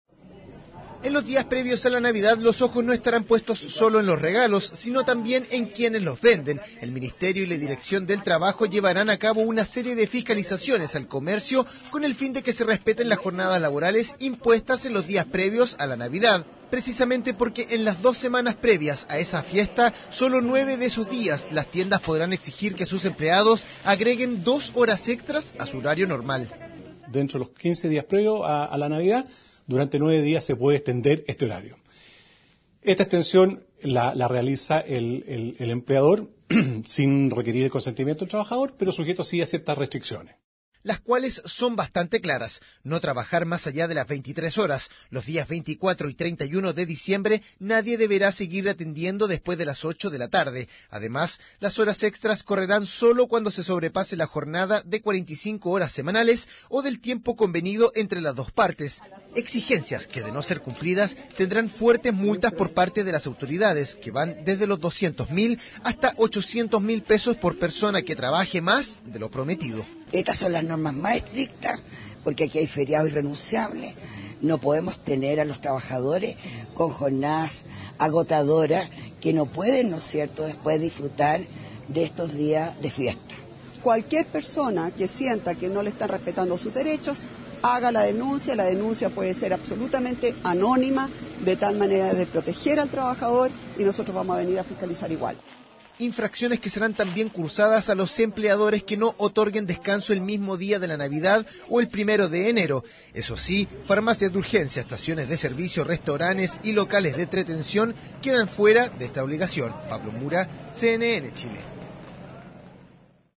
Los horarios de los trabajadores del comercio y las obligaciones de los empleadores en Navidad
El Ministerio y la Dirección del Trabajo llevarán a cabo una serie de fiscalizaciones al comercio con el fin de que se respeten las jornadas laborales impuestas en los días previos a la Navidad.